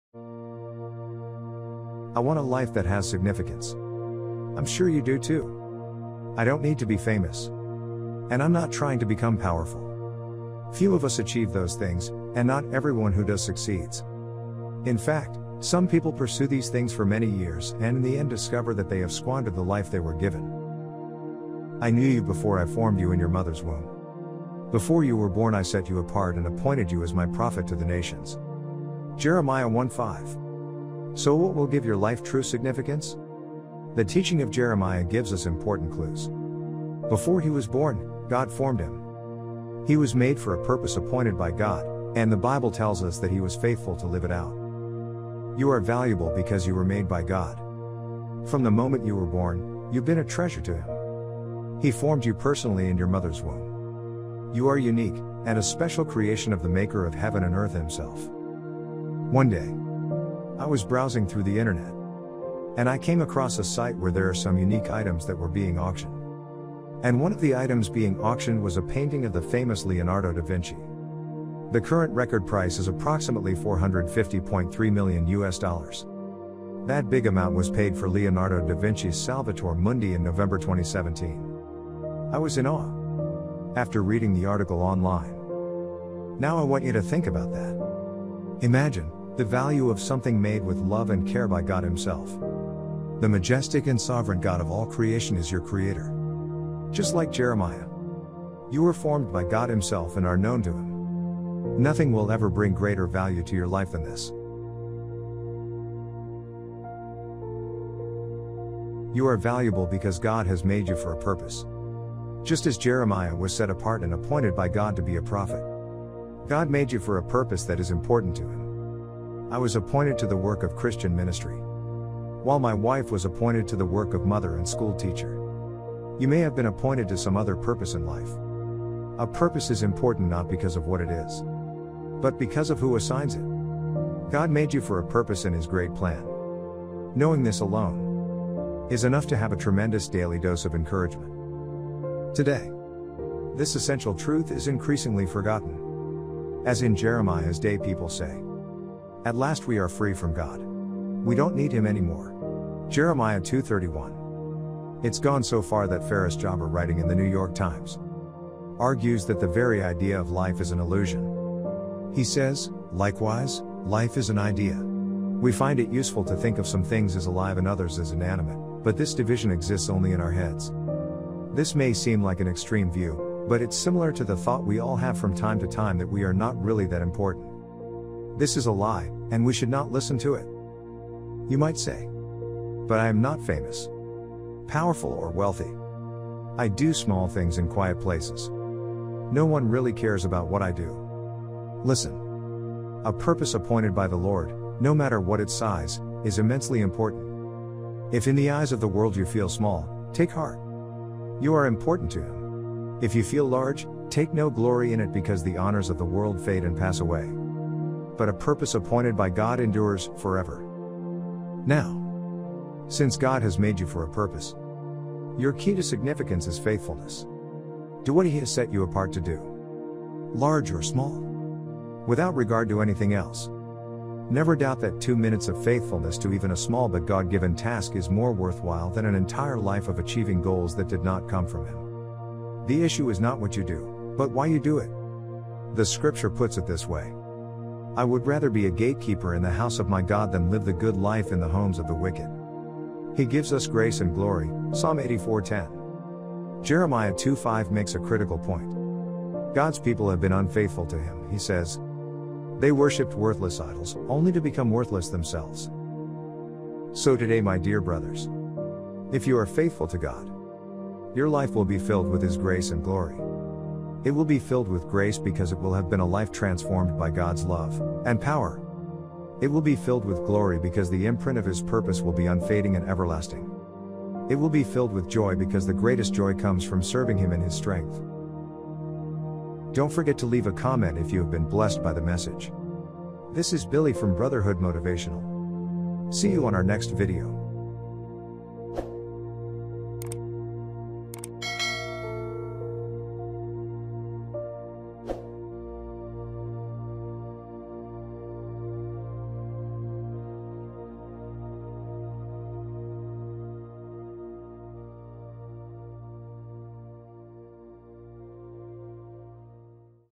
HOW TO LIVE A SIGNIFICANT LIFE !!
God has made you for a purpose, your key to significance is faithfulness. Do what he has set you apart to do, large or small, without regard to anything else.

Never doubt that two minutes of faithfulness to even a small but God-given task is more worthwhile than an entire life of achieving goals that did not come from him. The issue is not what you do but why you do it. The Scripture puts it this way I would rather be a gatekeeper in the house of my God than live the good life in the homes of the wicked [...] He gives us grace and glory" (Psalm 84:10b, 11b).